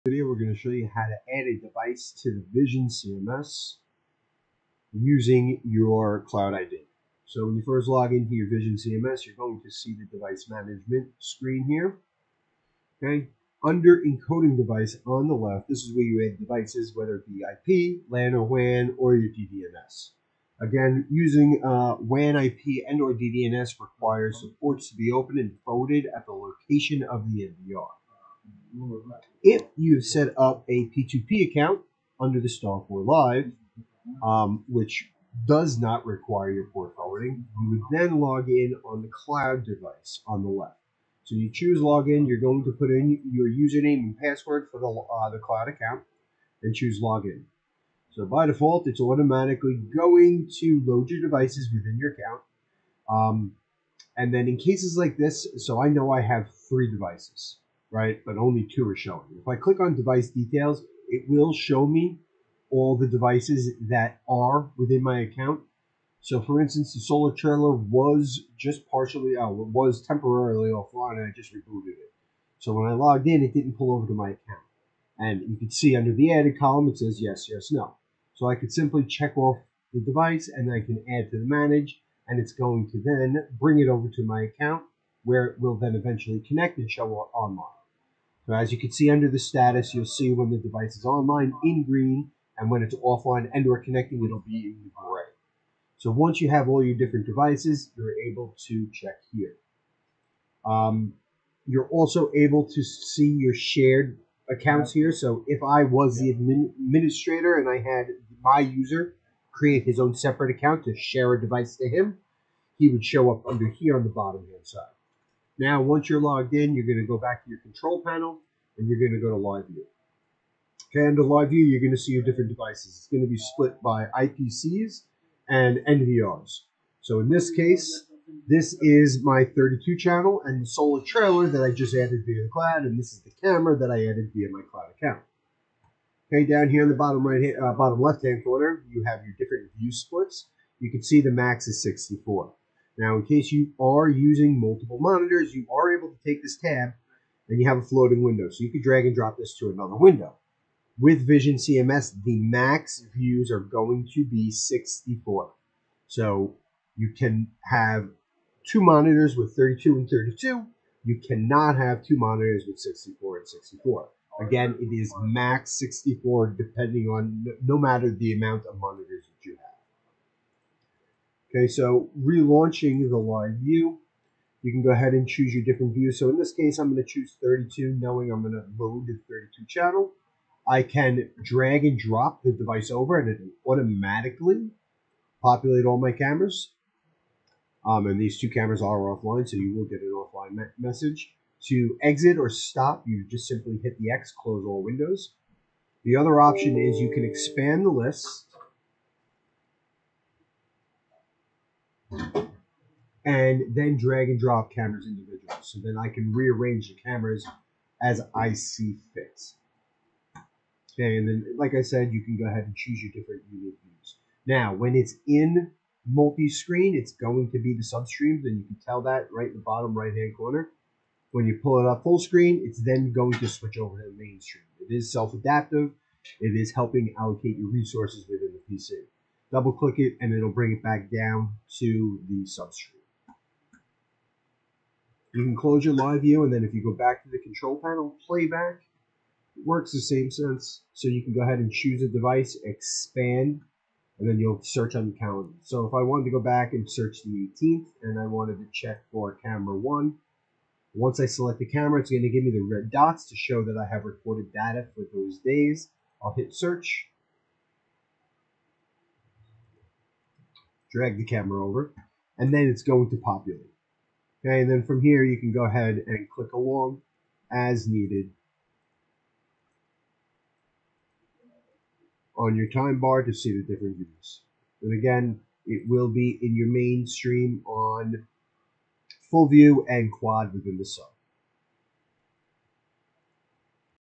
Vision CMS: Logging into You Cloud Account & Using Live View and Playback
Vision CMS: Logging into Your Cloud Account & Using Live View and Playback